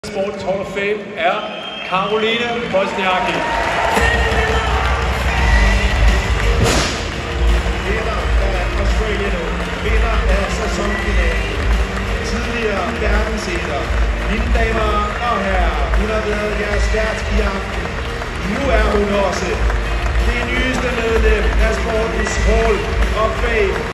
Caroline Wozniacki optaget i sportens Hall of Fame
Den tidligere tennisspiller, Caroline Wozniacki, blev lørdag den 7. januar 2023 optaget i sportens Hall of Fame ved det store Sport 2022-show i Jyske Bank Boxen. Se her øjeblikket, hvor H.K.H Kronprins Frederik offentliggører det.